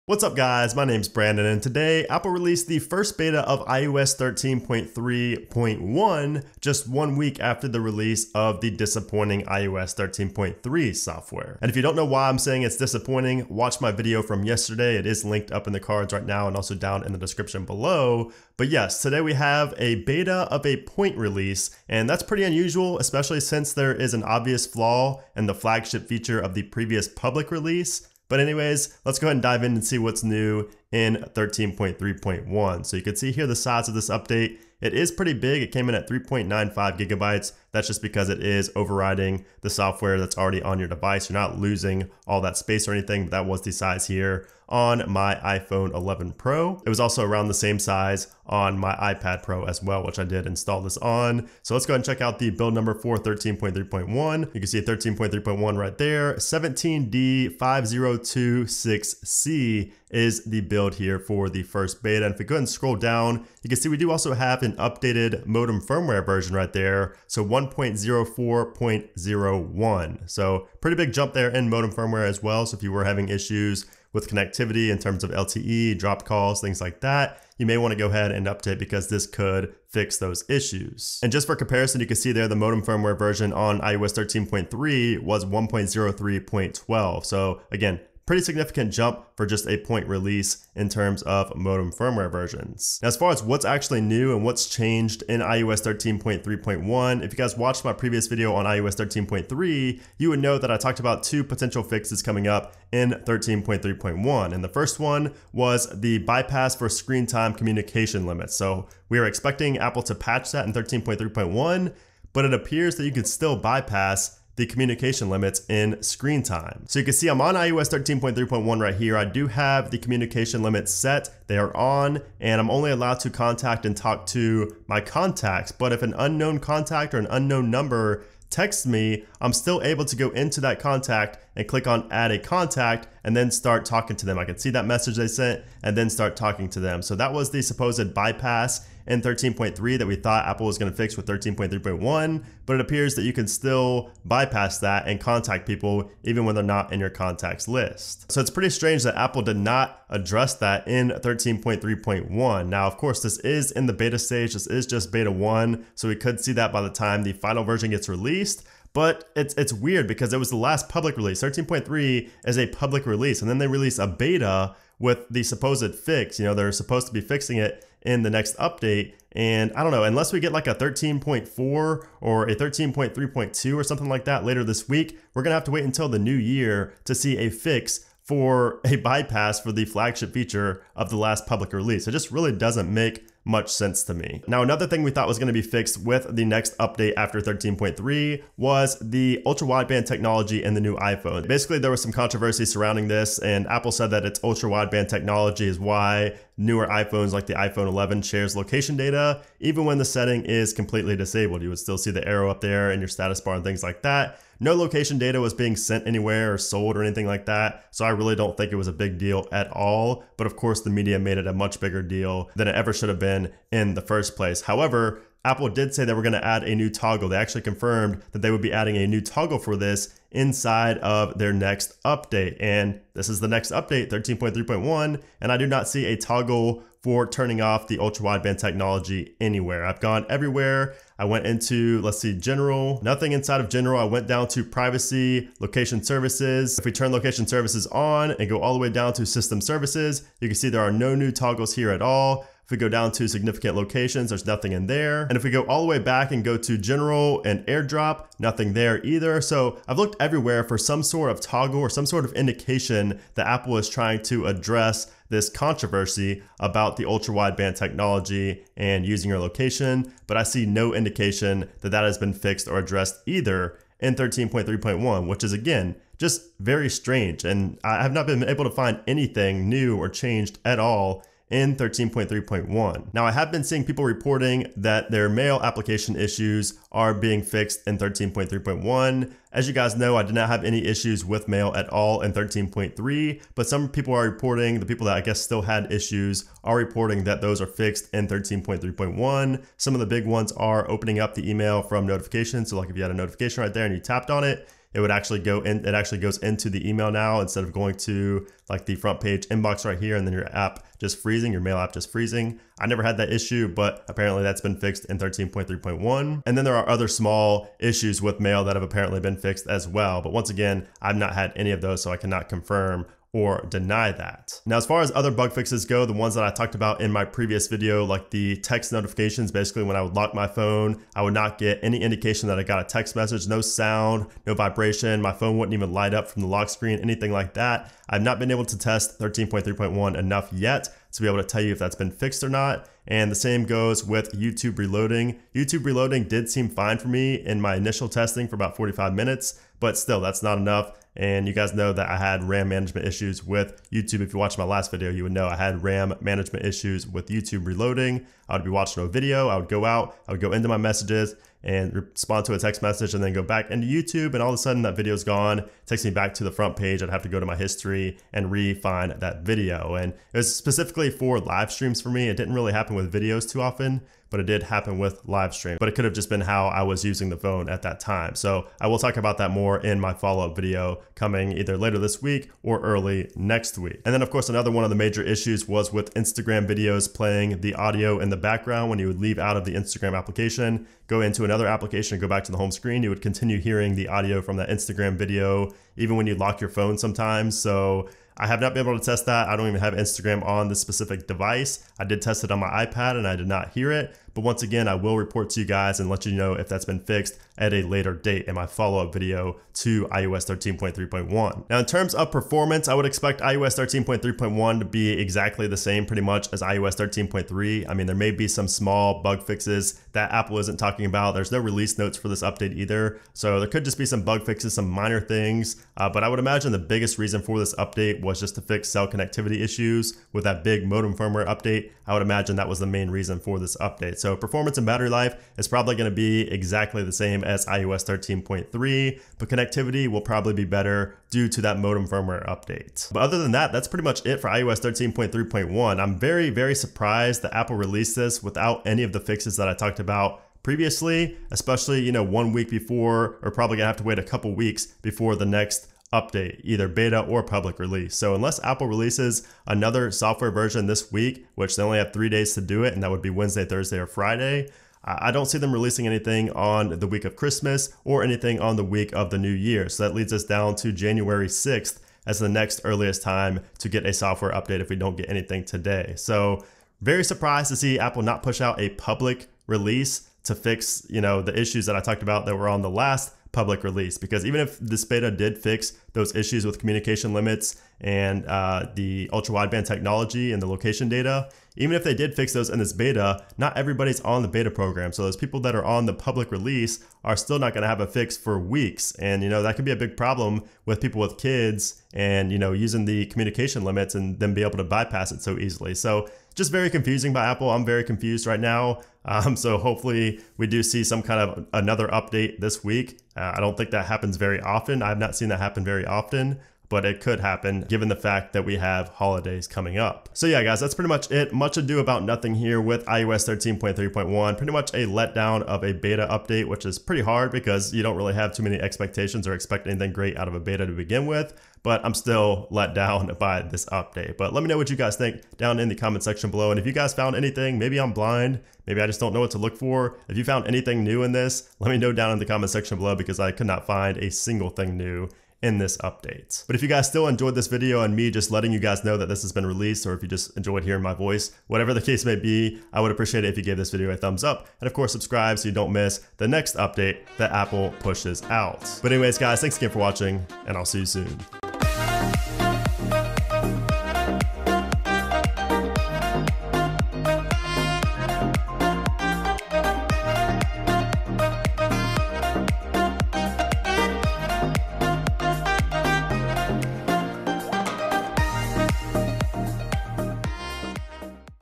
iOS 13.3.1 Beta 1 Released - What's New?
iOS 13.3.1 Released - What's New? | iOS 13.3.1 Changes & Features (Review)

Today, Apple released iOS 13.3.1 beta 1 to registered developers and in this video we cover what's new in the software.

Will this 13.3.1 update fix the ultrawideband/location services issue? Will it patch the screen time communication limit bypass? And what else will be improved with this latest software update? Let's find out.

Did you update to iOS 13.3.1 yet? Or are you staying on iOS 13.3?

▶️ Become a 🔥VIP member 🔥 to see videos early, get VIP support + other perks

▬ STAY UP-TO-DATE! ▬